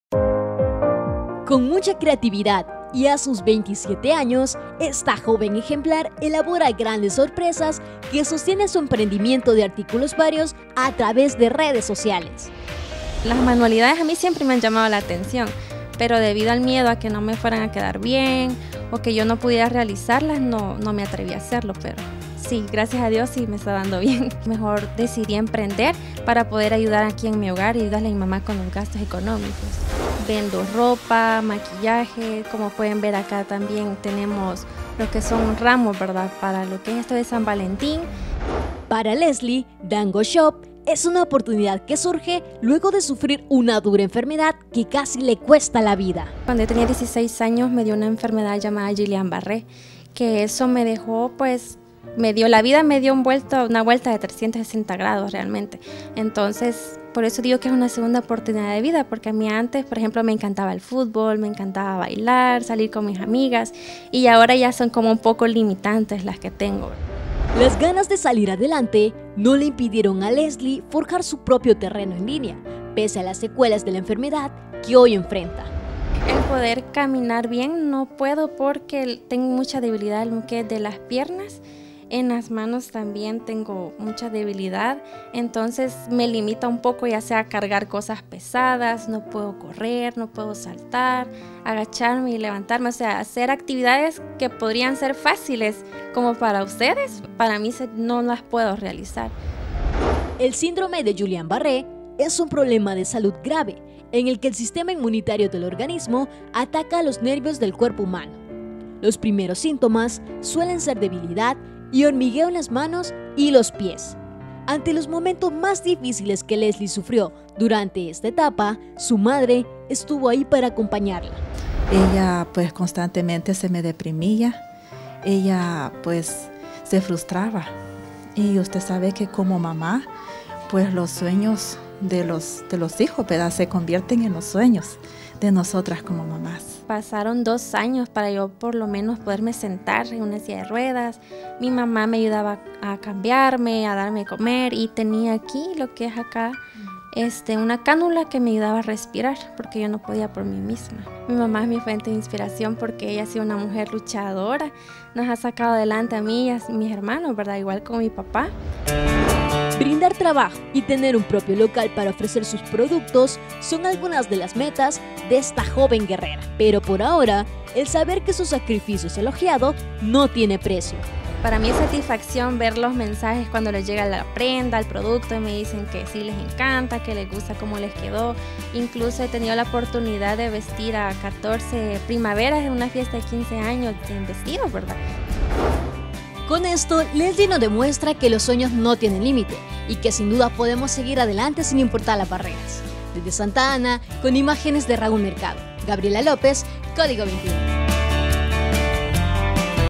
Lesly Orellana logró emprender pese a que una enfermedad que casi le arrebata su vida
🙌🏻A pesar de los obstáculos que la vida pone, Gloria Lesly Orellana logró incursionarse en el mundo del emprendimiento pese a que una enfermedad que casi le arrebata su vida. “Dango Shop” ha sido el nombre que se ha posicionado.

Entérate del sueño de esta hermosa mujer 😍✨